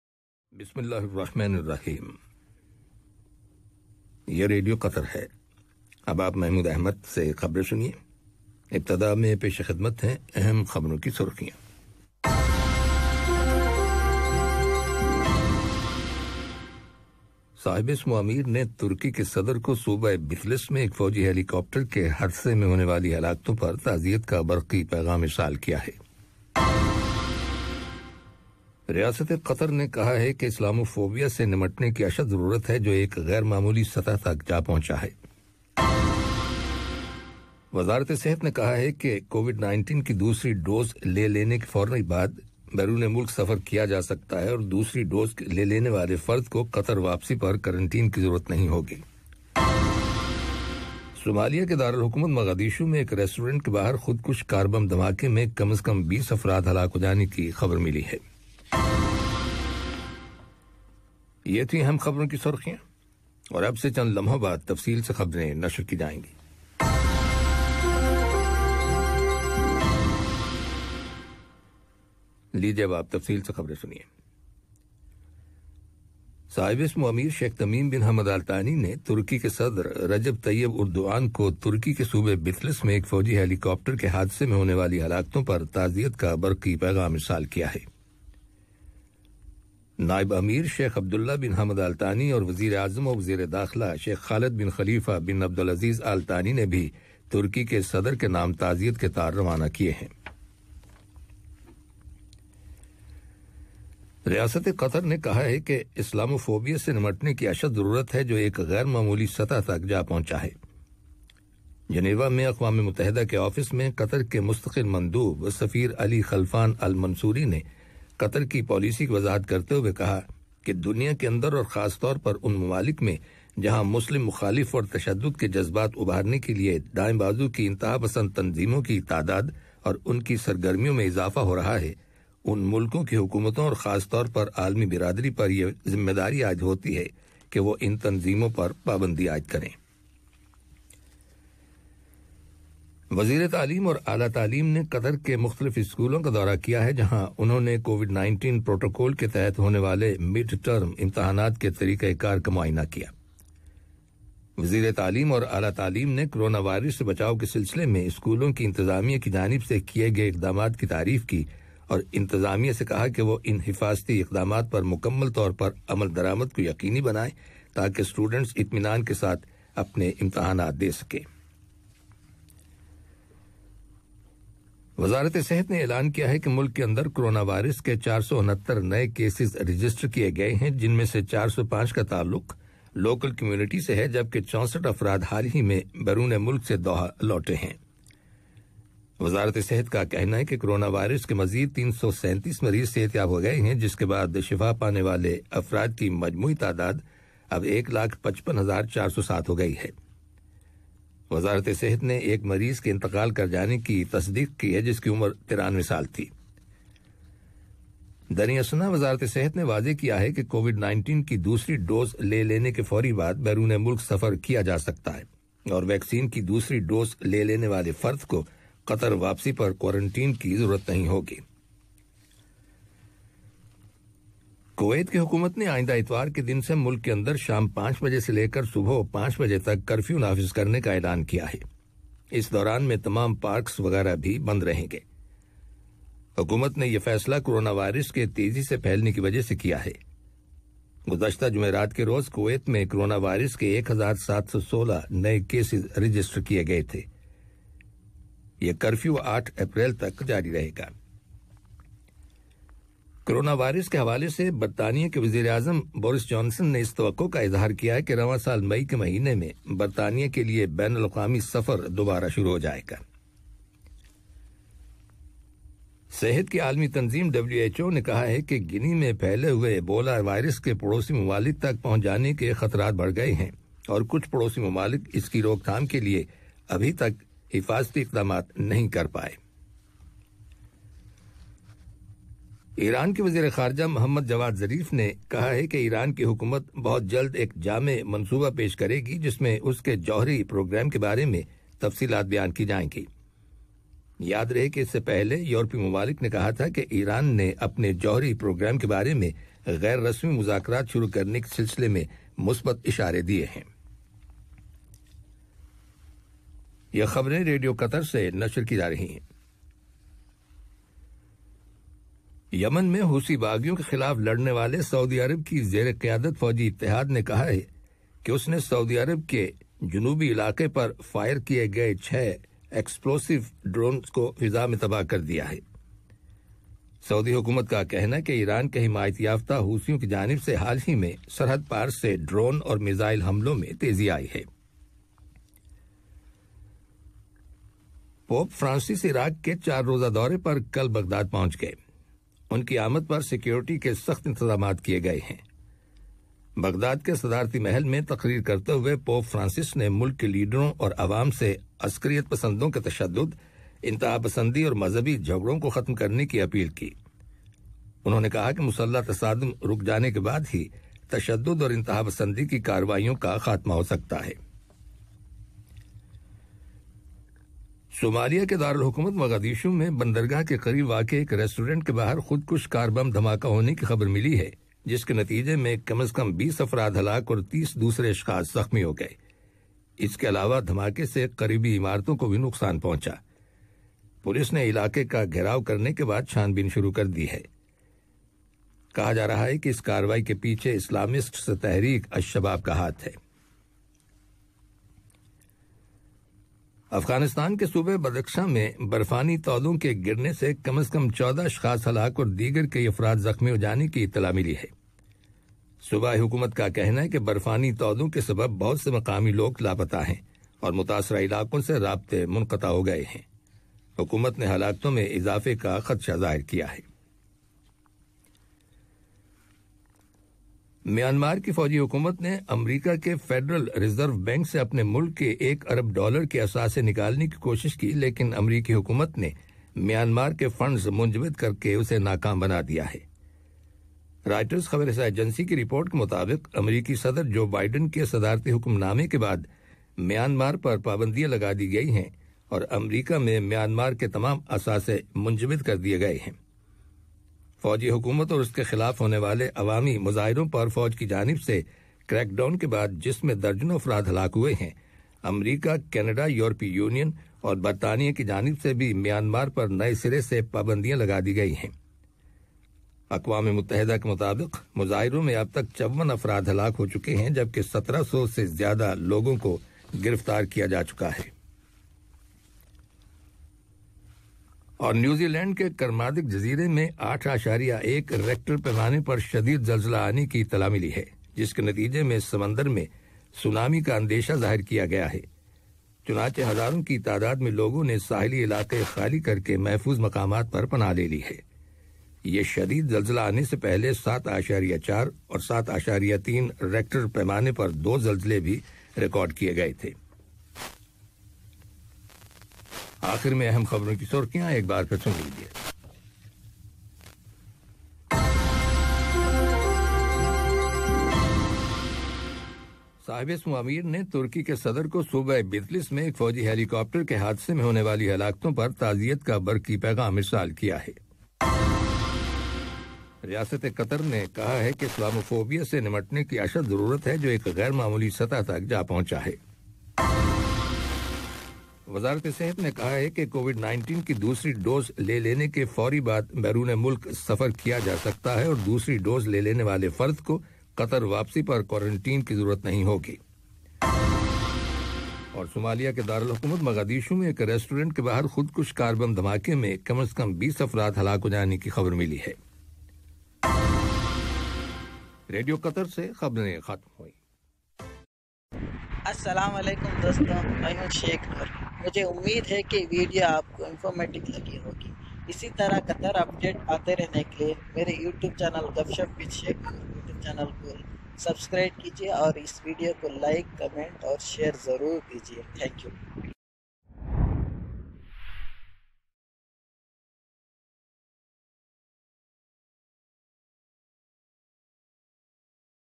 Qatar news today HH the Amir send a cable of condolences to to President of the republic of turkey
HH the Amir send a cable of condolences to to President of the republic of turkey on the victims of a military helicopter crash in the province of bitlis, wishing a speedy recovery for the injured..,!

Only 8% of the applications from private schools for a fee hike have been approved, an official of the Ministry of Education and Higher Education said...!

Myanmar's junta lost a tug of war over leadership of its UN mission in New York and the United States unveiled new sanctions targeting military conglomerates after the deaths of dozens of civilians protesting against last month's coup...!

One of the strongest earthquakes to hit the South Pacific in modern history triggered tsunami warnings across the ocean and forced thousands in NewZealand to evacuate coastal areas Friday..!